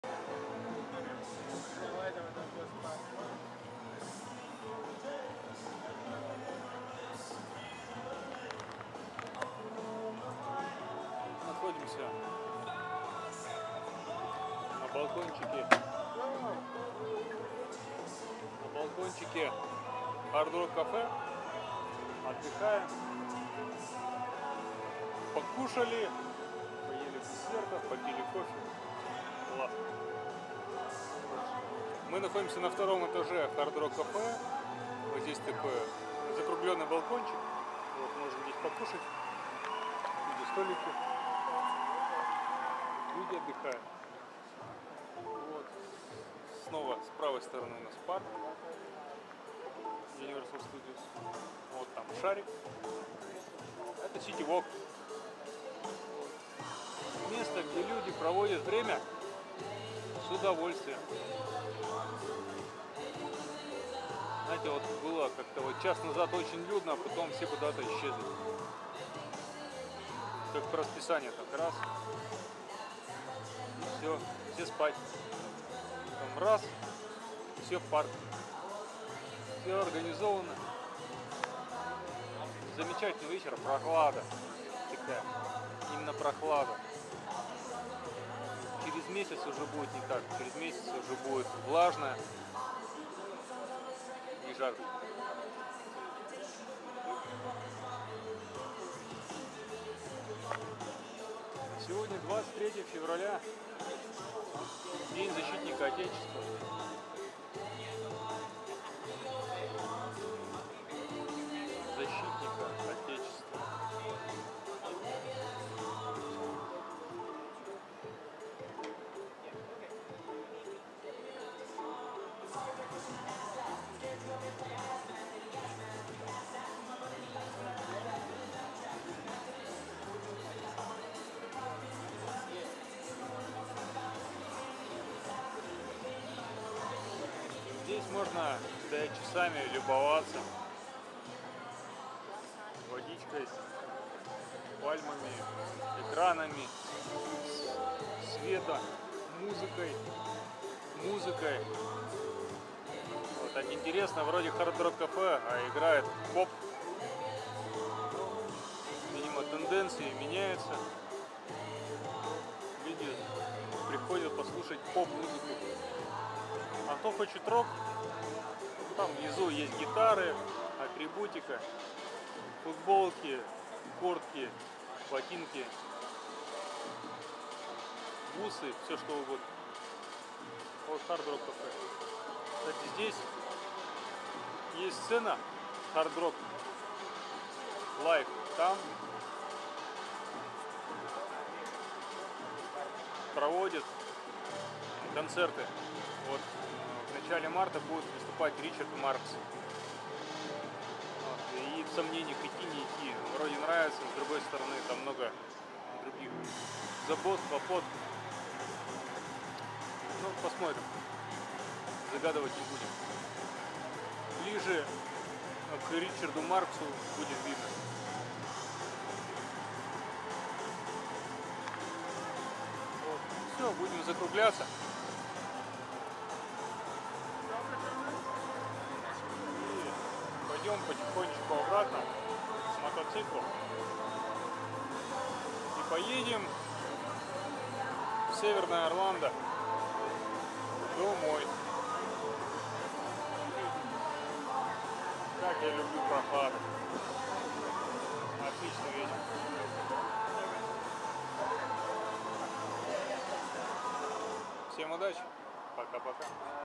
№75. Парк Universal Studios, Orlando, Florida. (Part I)
Thursday, February 23rd, 2017
View on the 2nd floor of Hard Rock Cafe.